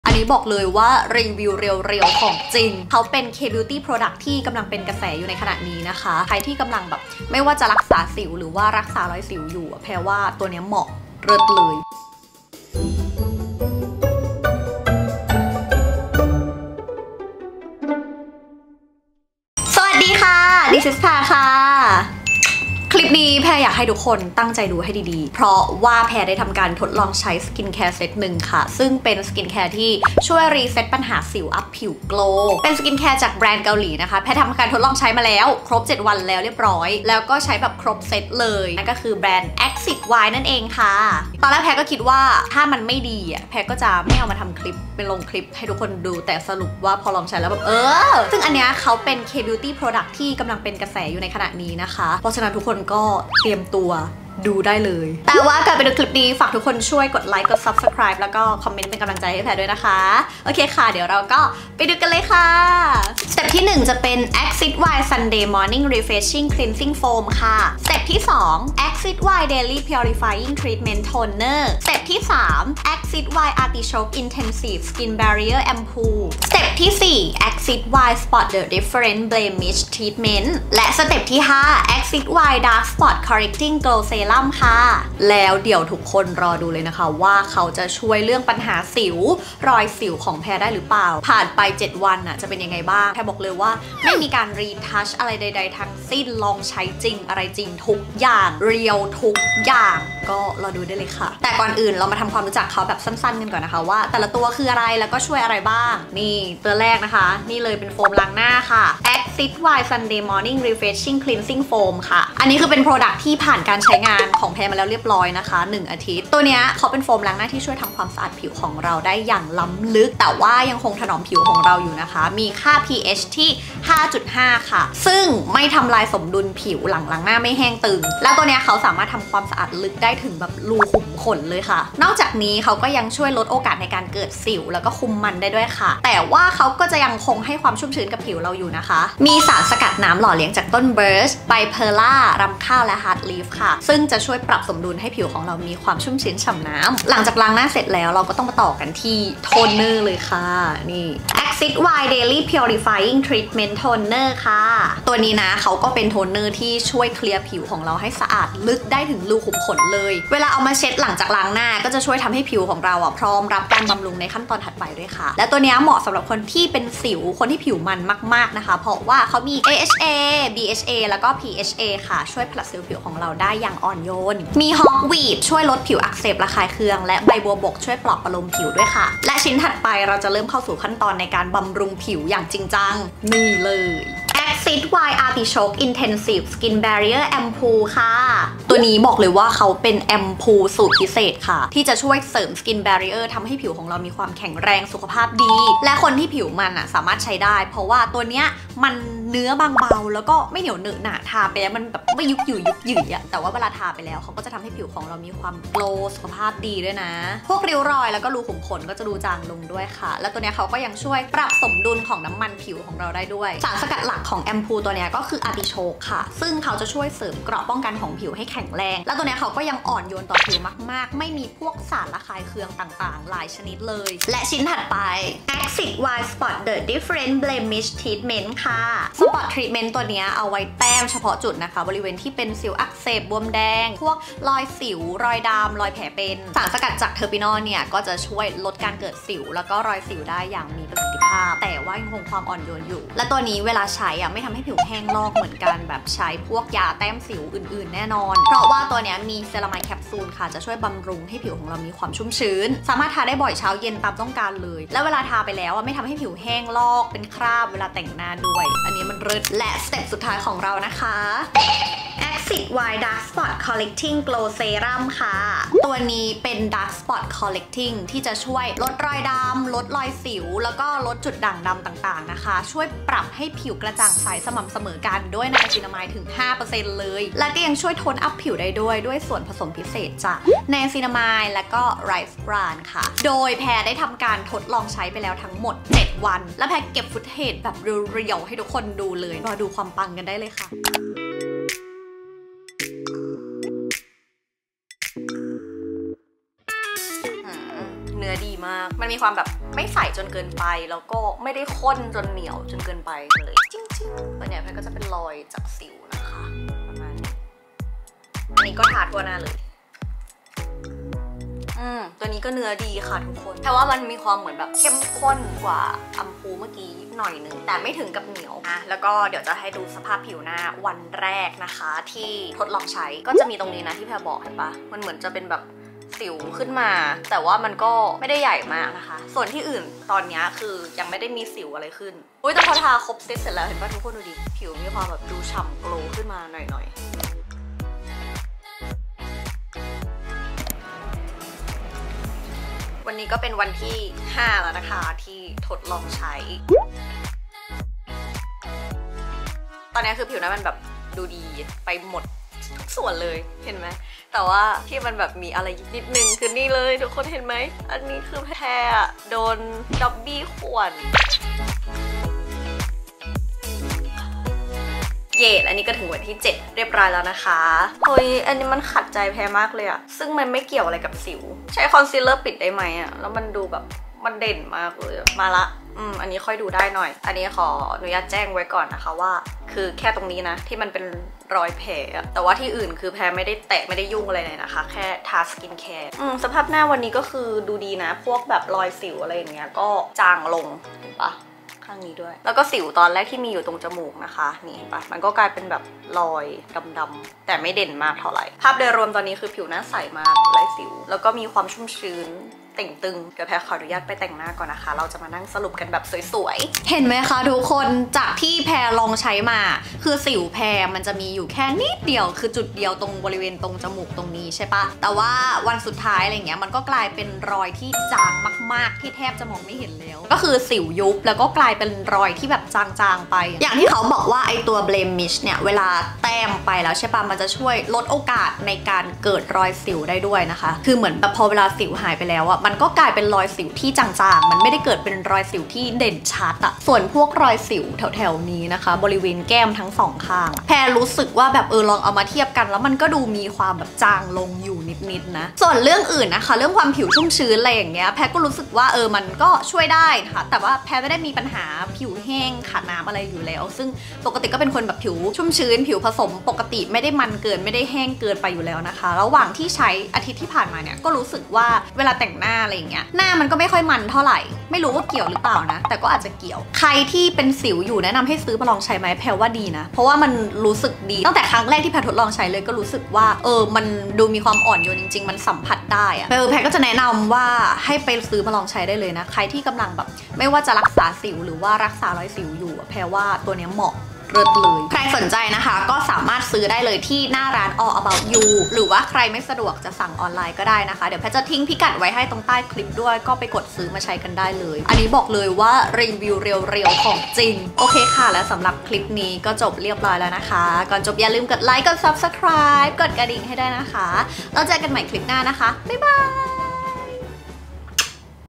รีวิวใช้จริง สกินแคร์รักษาสิว Axis Y ครบเซ็ต ! สิวยุบไวมั้ย? รอยสิวดูจางลง? | This is Pear
✨คลิปนี้แพรมารีวิวสกินแคร์เซ็ตรักษาสิวจากแบรนด์ AXIS-Y ลองใช้เป็นเซ็ตเลย มาดูกันว่าเค้าจะช่วยเรื่องสิว รอยสิวได้ดีแค่ไหน?

📍พิกัด ซื้อได้ที่ All About You ทุกสาขา
หรือ Shopee, Lazada, Watsons, Eveandboy, Konvy, Beautrium,Multy,KIS

หรือช่องทางออนไลน์ที่

What I use:
AXIS-Y Sunday Morning Refreshing Cleansing Foam
AXIS-Y Daily Purifying Treatment Toner
AXIS-Y Artichoke Intensive Skin Barrier Ampoule
AXIS-Y Spot The Difference Blemish Treatment
AXIS-Y Dark Spot Correcting Glow Serum

🌷E Q U I P E M E N T
SONY ZV-1
CAPCUT